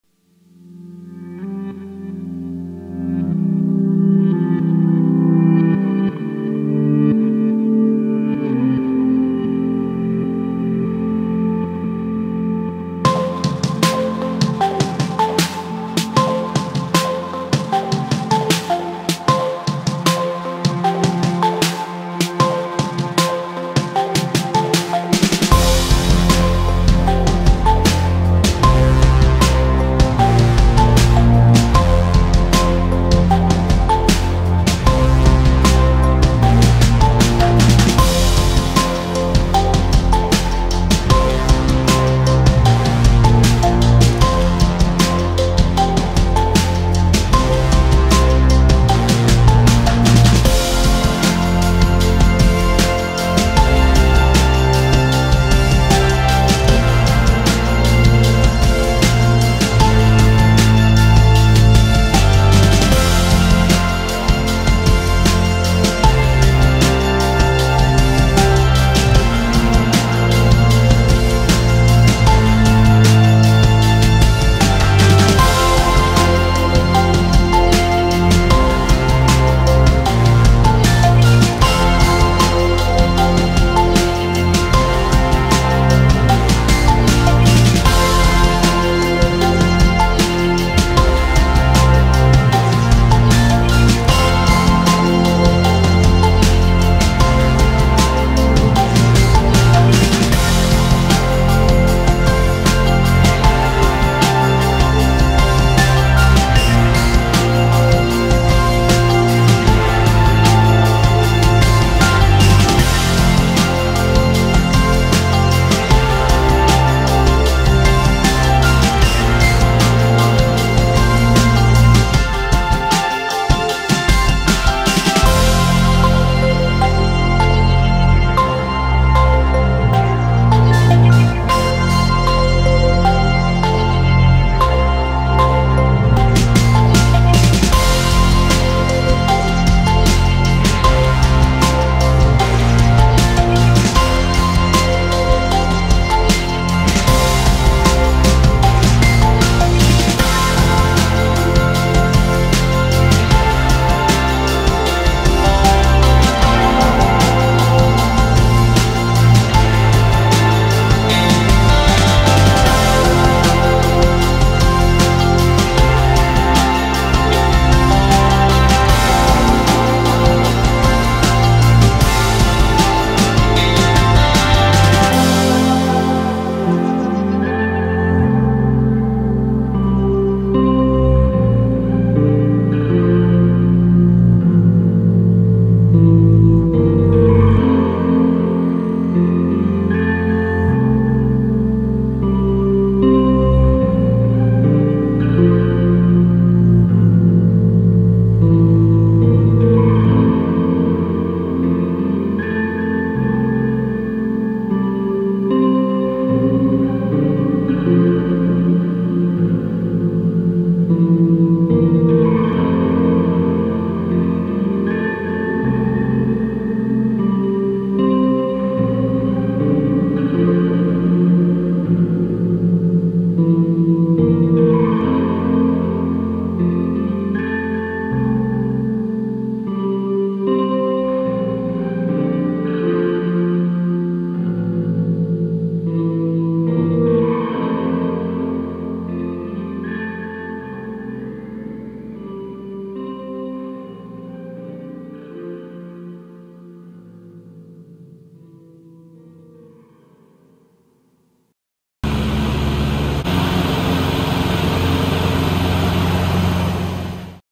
Saradise
A serene aerial tour of the 3 villages - Davistown, Saratoga and Yattalunga.
Central Coast, NSW, Australia